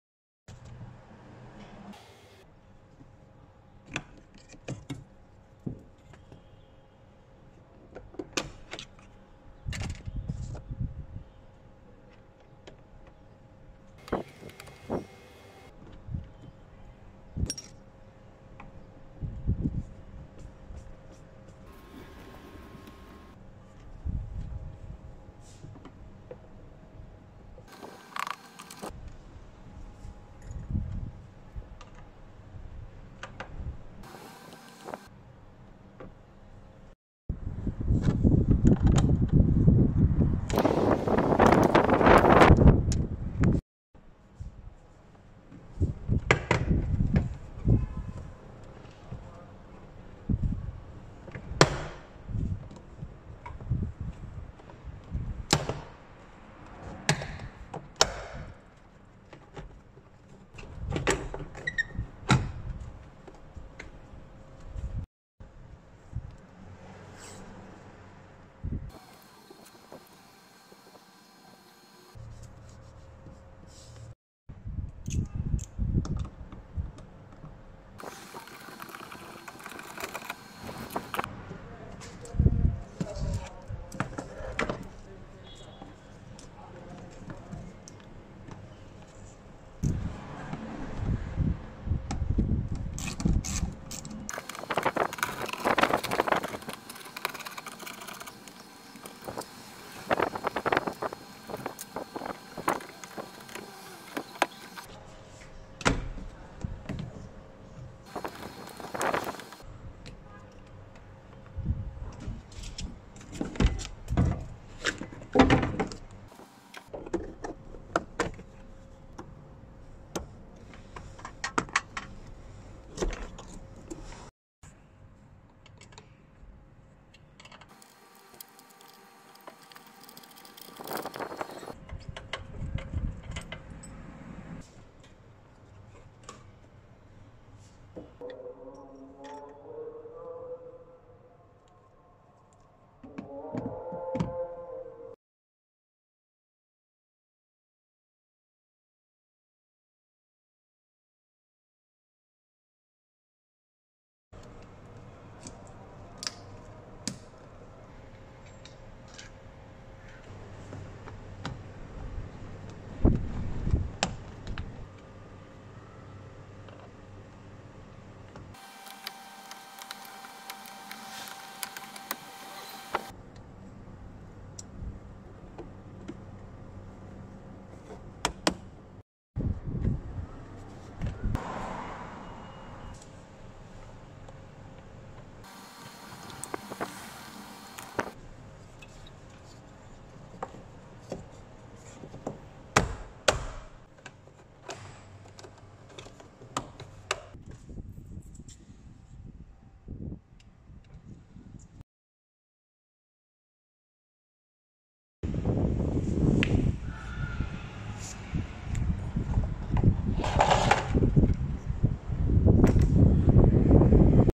Jeep Wrangler 2012 Rear window regulator replacement
DIY:  2012 Jeep Wrangler rear window regulator  replacement